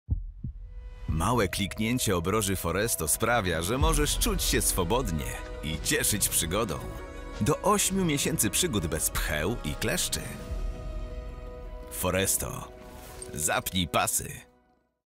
Obroża Foresto dla kotów – zapnij pasy!
Kiedy wybierasz się na ściankę wspinaczkową, robisz klik – i kask ochronny jest na swoim miejscu. Podobnie Twój kot może być chroniony przed pchłami i kleszczami – wystarczy jedno kliknięcie obroży Foresto (chroni do 8 miesięcy). To jak, zapinamy pasy?

Przed użyciem zapoznaj się z treścią ulotki dołączonej do opakowania.